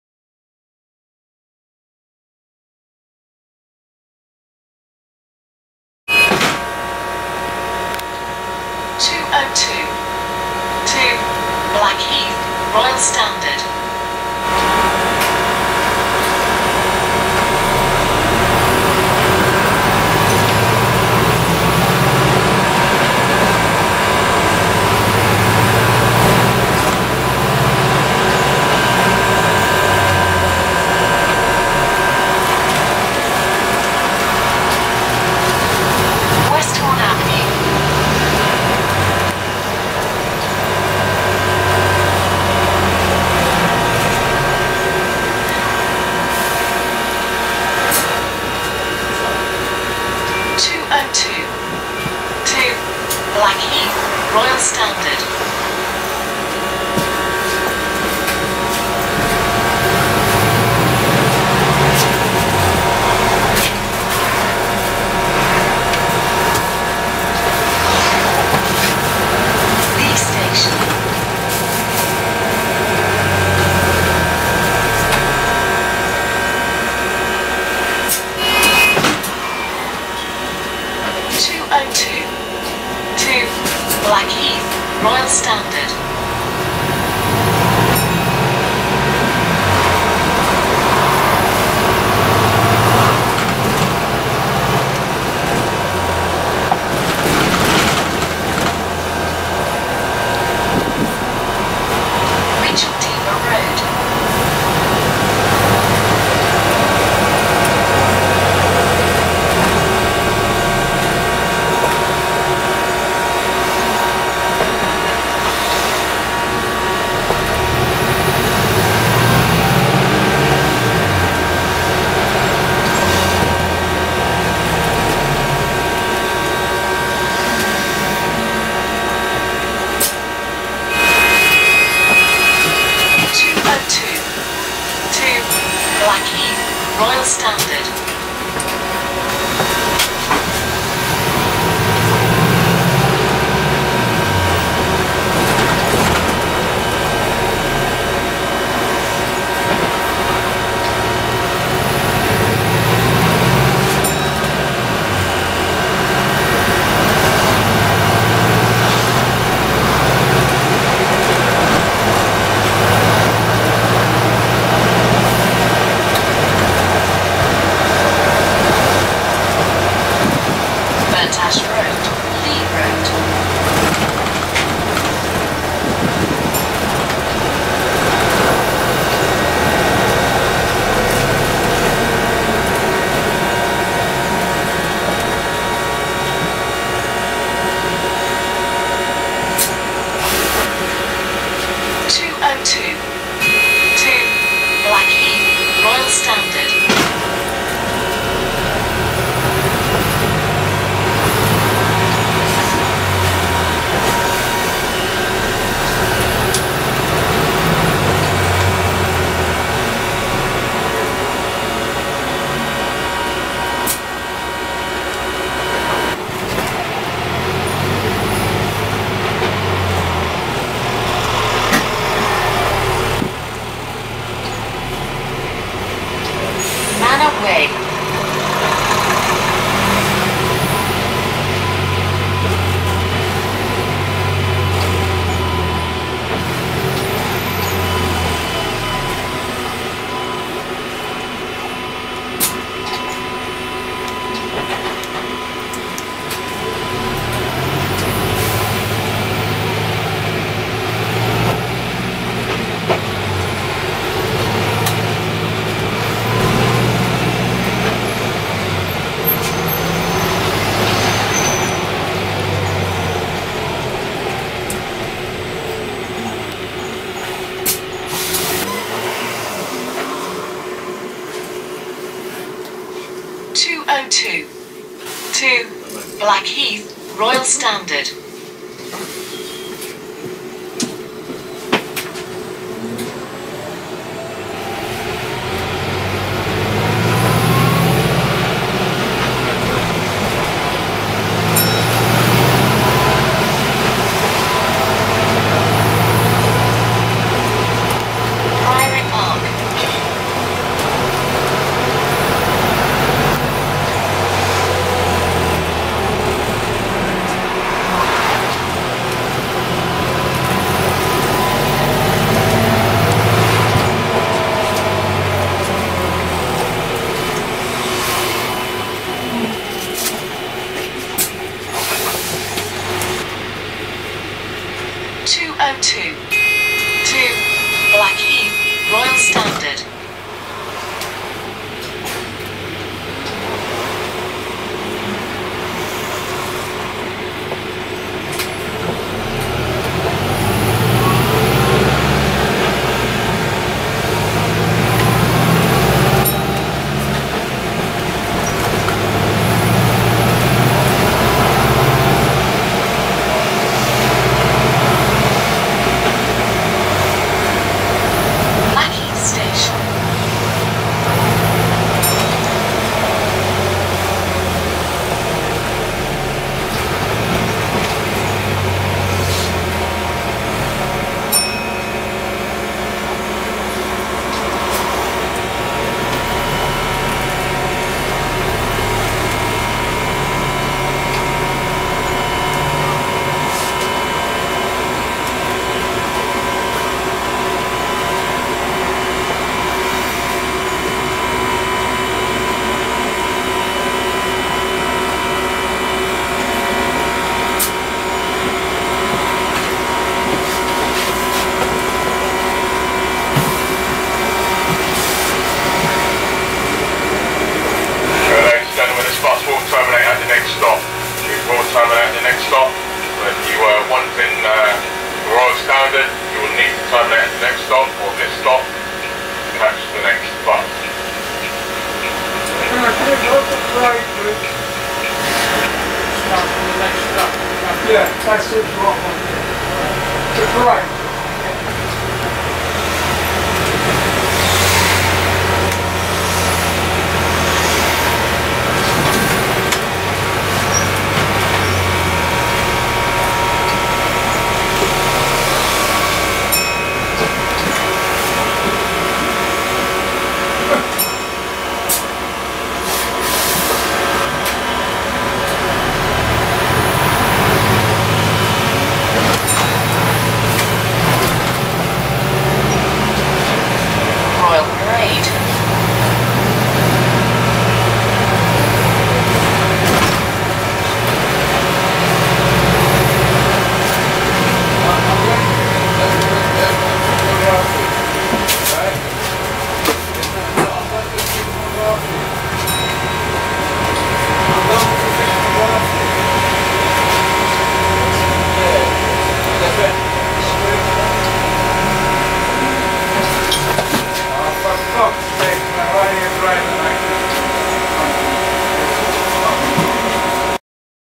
Journey On The 202 (WHV42 BP15OLN) Volvo B5L Hybrid 10.5m/Wright Eclipse Gemini 3
This bus was serving Route 202 To Blackheath, Royal Standard
Route 202: Crystal Palace - Blackheath, Royal Standard
Operated By: Go Ahead London Metrobus
London General
Tuesday, August 25, 2015 at 12.43